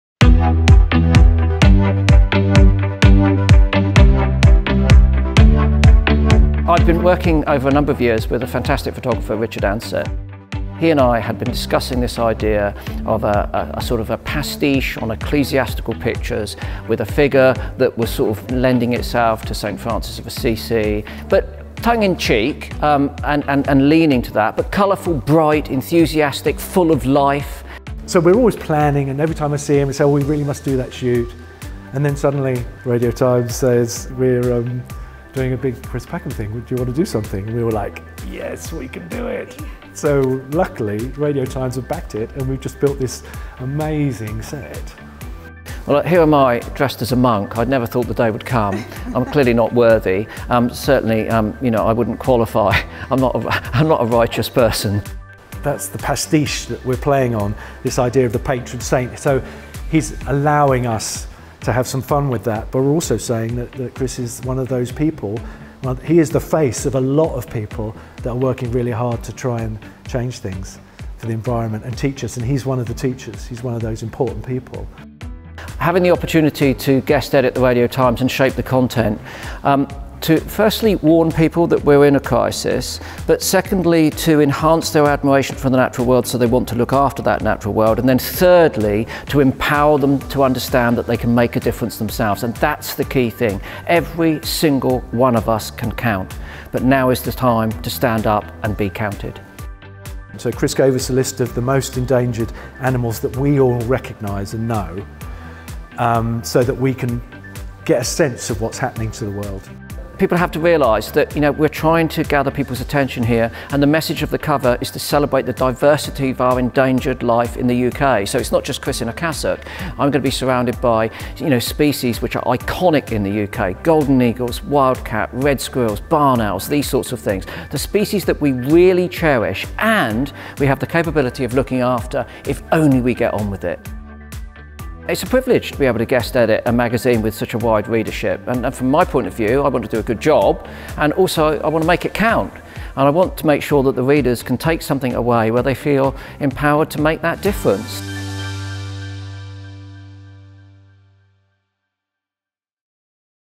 Go behind-the-scenes with Chris Packham on Radio Times' exclusive shoot
This week's Earth Day issue has a very special cover featuring the Springwatch star.

The Earth Day special issue of Radio Times magazine is out on Tuesday 16th April 2024.